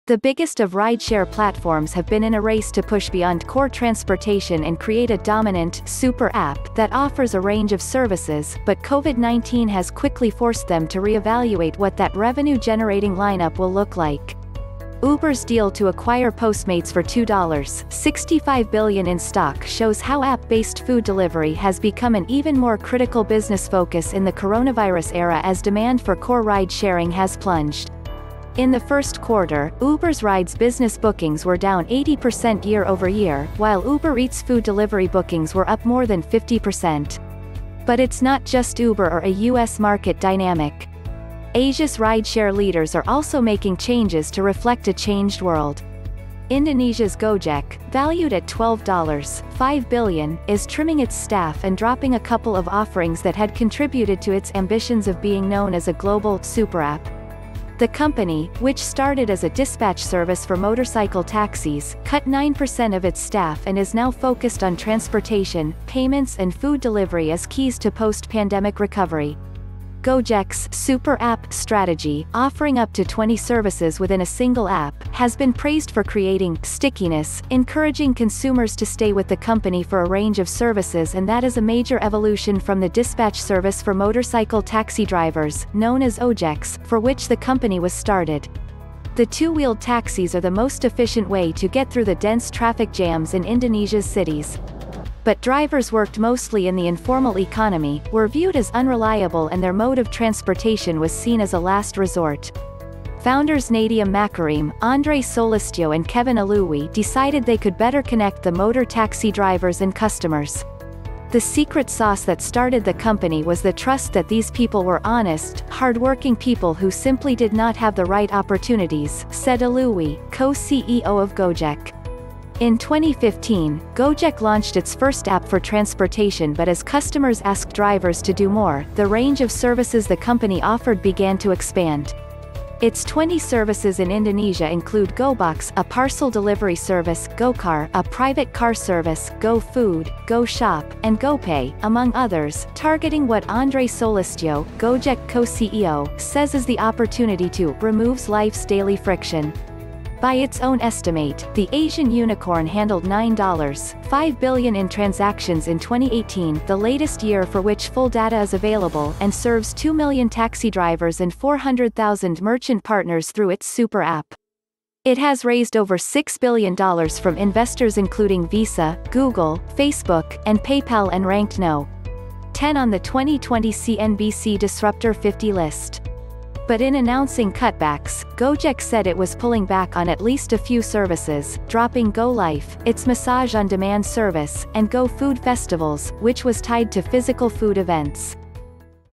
From Uber to Asia the pandemic is remaking the world's 'super app' race
The biggest of rideshare platforms have been in a race to push beyond core transportation and create a dominant 'super app' that offers a range of services, but Covid-19 has quickly forced them to reevaluate what that revenue-generating lineup will look like. Uber's deal to acquire Postmates for $2. 65 billion in stock shows how app-based food delivery has become an even more critical business focus in the coronavirus era as demand for core ride-sharing has plunged. In the first quarter, Uber's rides business bookings were down 80% year over year, while UberEats food delivery bookings were up more than 50%. But it's not just Uber or a U. S. market dynamic. Asia's rideshare leaders are also making changes to reflect a changed world. Indonesia's Gojek, valued at $12. 5 billion, is trimming its staff and dropping a couple of offerings that had contributed to its ambitions of being known as a global ''superapp. '' The company, which started as a dispatch service for motorcycle taxis, cut nine percent of its staff and is now focused on transportation, payments and food delivery as keys to post-pandemic recovery. Gojek's ''super app'' strategy, offering up to 20 services within a single app, has been praised for creating ''stickiness'' — encouraging consumers to stay with the company for a range of services and that is a major evolution from the dispatch service for motorcycle taxi drivers, known as ojeks, for which the company was started. The two-wheeled taxis are the most efficient way to get through the dense traffic jams in Indonesia's cities. But drivers worked mostly in the informal economy, were viewed as unreliable and their mode of transportation was seen as a last resort. Founders Nadiem Makarim, Andre Soelistyo and Kevin Aluwi decided they could better connect the motor taxi drivers and customers. ''The secret sauce that started the company was the trust that these people were honest, hardworking people who simply did not have the right opportunities, '' said Aluwi, co-CEO of Gojek. In 2015, Gojek launched its first app for transportation but as customers asked drivers to do more, the range of services the company offered began to expand. Its 20 services in Indonesia include Gobox (a parcel delivery service), GoCar (a private car service), GoFood, GoShop, and GoPay, among others, targeting what Andre Soelistyo, Gojek co-CEO, says is the opportunity to ''removes life's daily friction. ''By its own estimate, the Asian unicorn handled $9. 5 billion in transactions in 2018 (the latest year for which full data is available) and serves two million taxi drivers and 400, 000 merchant partners through its super app. It has raised over $6 billion from investors including Visa, Google, Facebook, and Paypal and ranked No. 10 on the 2020 CNBC Disruptor 50 list. But in announcing cutbacks, Gojek said it was pulling back on at least a few services: dropping GoLife, its massage on-demand service, and GoFood Festivals, which was tied to physical food events.